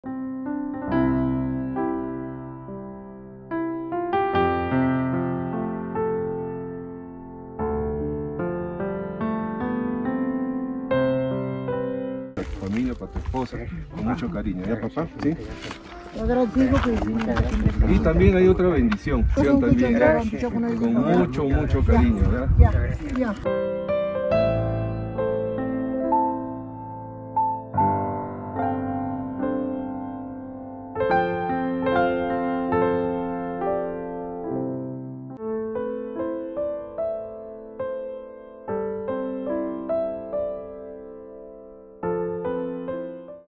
Vive en la helada Cordillera Andina en extrema pobreza y abandono / ancianito quechua-hablante
Ancianito quechua-hablante vive en las heladas montañas de los Andes de Apurímac en situación de extrema pobreza y necesidad... Sobre los 3,400 msnm enfrenta temperaturas con intenso frío... Logramos llegar hasta este lugar y recibimos su expresión de sorpresa y agradecimiento...  Coronel Juan Silva Bocanegra y Off The Trail Missions...